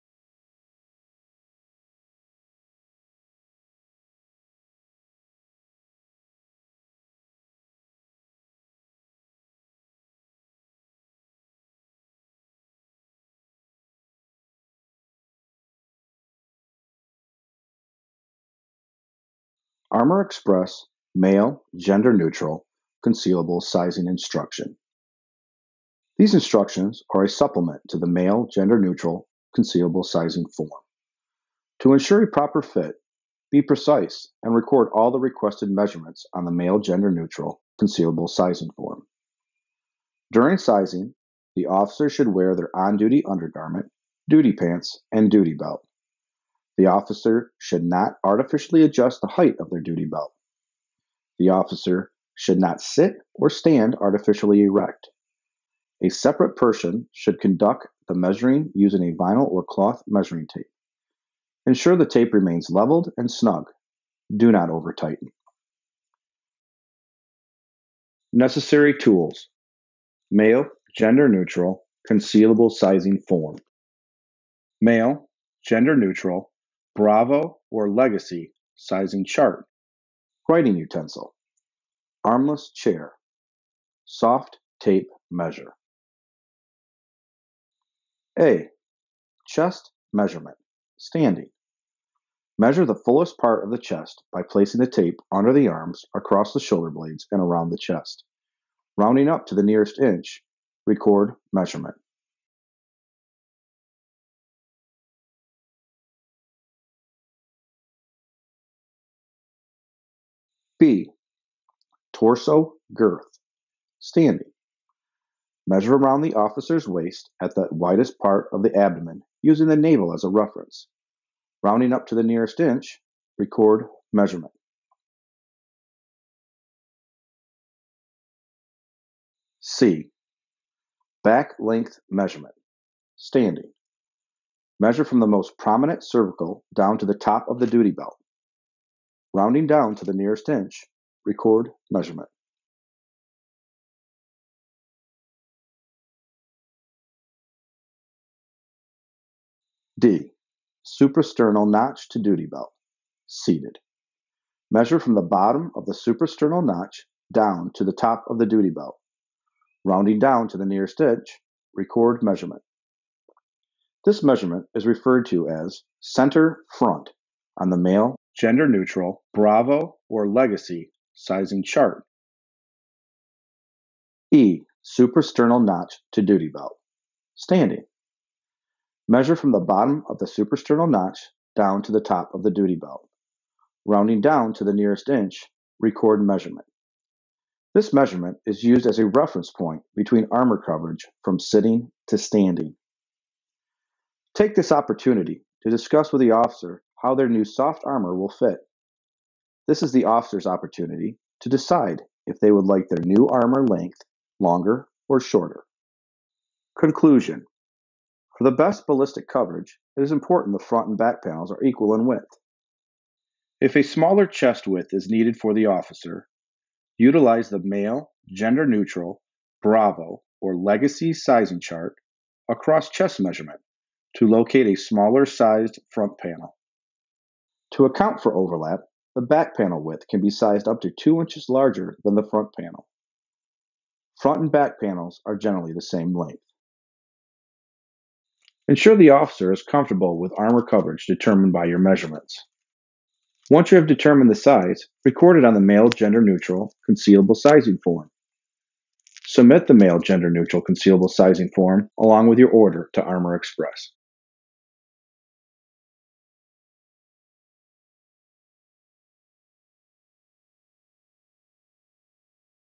Male/Gender-Neutral Concealable Sizing Instructions
These instructions are intended to aid an agency during the measurement process for their Armor Express concealable body armor.  These guidelines will help ensure proper armor fit and coverage. We are proud to serve those who protect us.